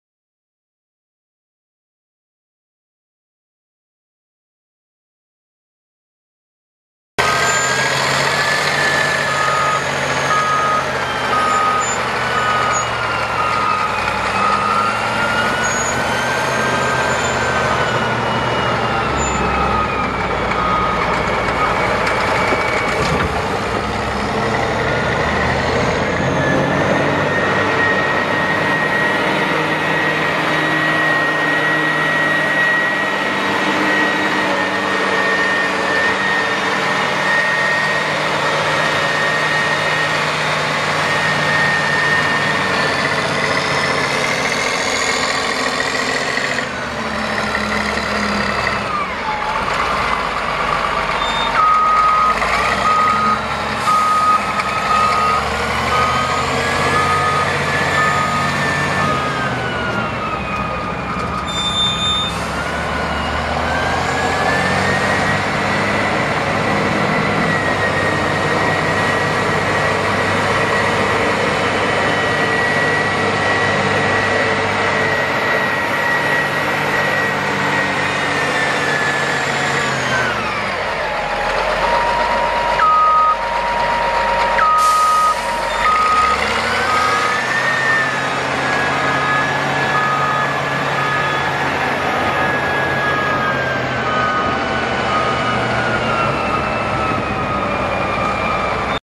1972 Caterpillar 980B wheel loader for sale | sold at auction May 10, 2012
SOLD! Heavy Construction and Aggregate Equipment Auction

1972 Caterpillar 980B wheel loader
3,591 hours on meter
Caterpillar 3306 diesel engine
260 HP
Cab with heater
Spade nose bucket with teeth
26.5-25 tires
51,500 lbs. base operating weight
Serial 89P1924

It was located in Versailles, MO, and we sold it to the highest bidder for $10,670.